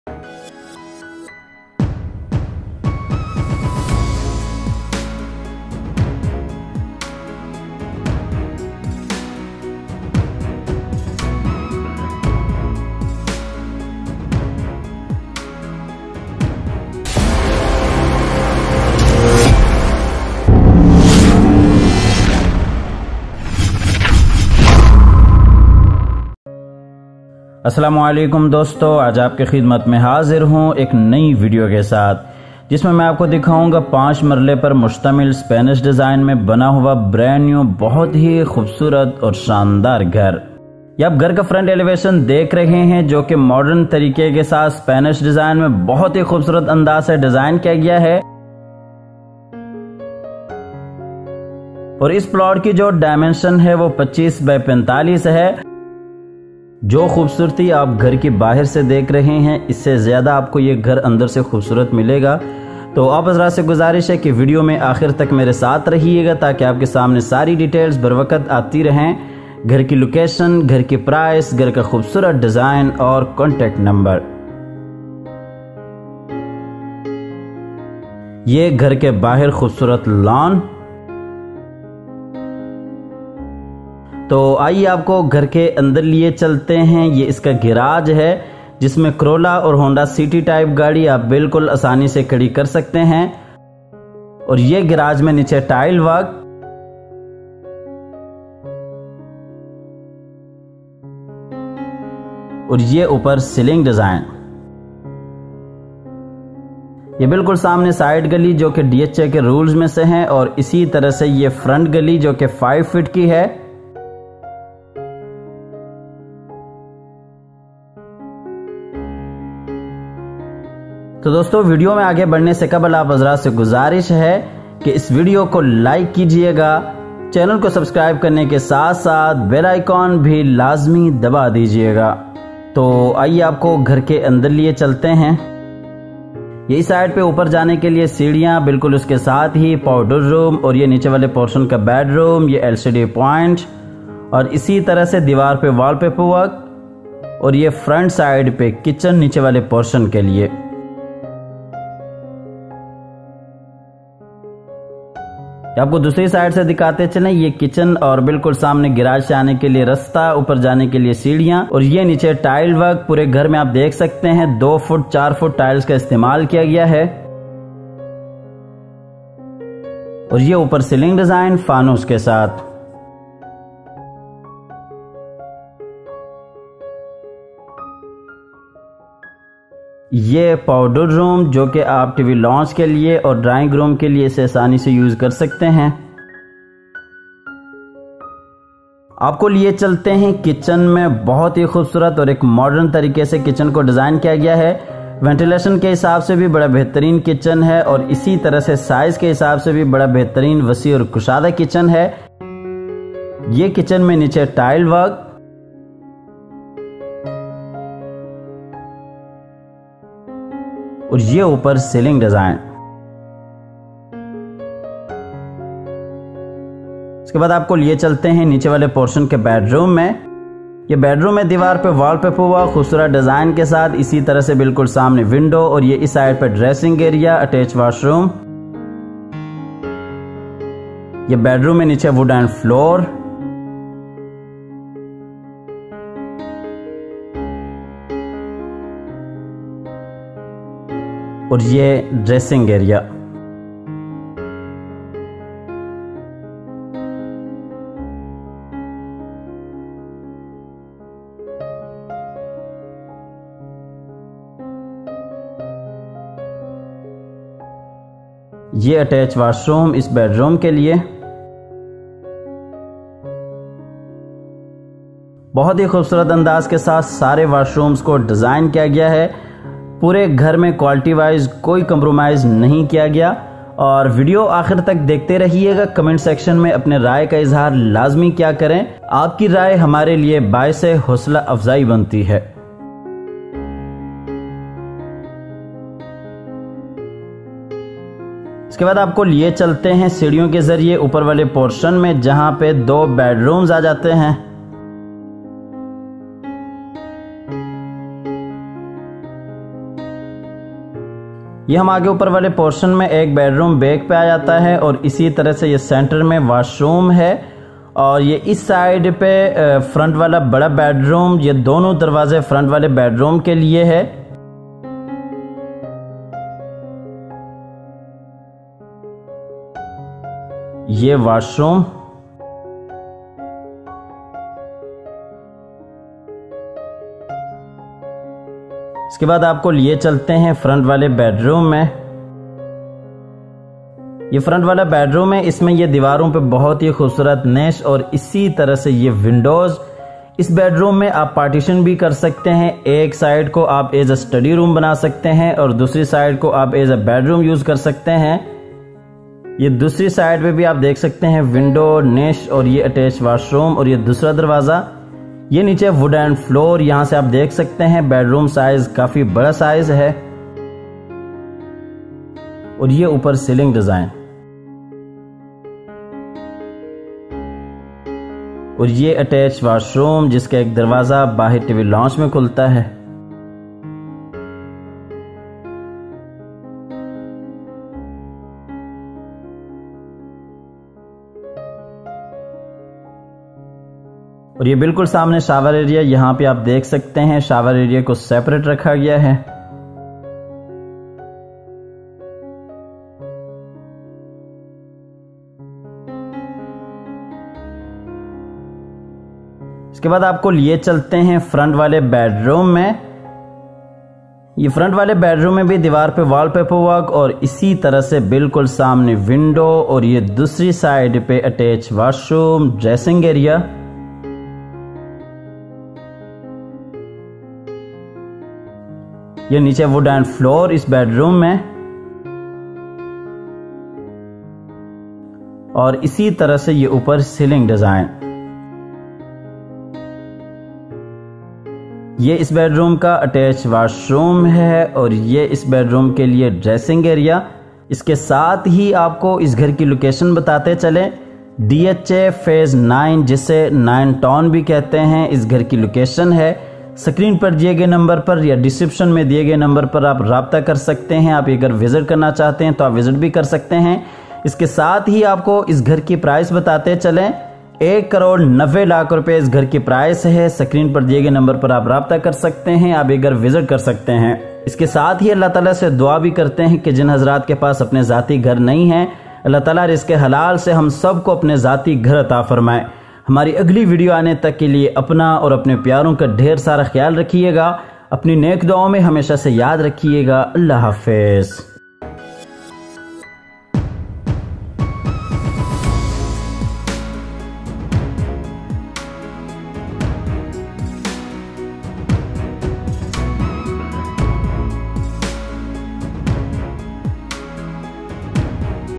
5 Marla Very Beautiful Spanish House 🏠 For Sale In DHA Lahore In Urdu/Hindi
5 Marla Brand New Beautiful Spanish House 🏡 for sale in DHA Phase 9 Town Lahore  | Urdu | Hindi / Al Ali Group
5 Marla Spanish House Design In Pakistan
5 Marla Spanish House For Sale In DHA Lahore

Ground-Flooor
                           ground floor car porch, drawing, TV Lounge, kitchen, and 1 bedroom with attached washroom.

First-Floor
                First floor Terrace, TV lounge and 2 bedroom with attached washroom.
0:00 Trailer
0:16 Intro
0:41 Front Elevation
1:27 Car Porch
2:01 Channel Subscribe
2:12 Drawing Room
3:09 Kitchen
3:36 Bedroom
4:47 Upr Portion
5:28 Bedroom
6:39 Bedroom
7:45 Dua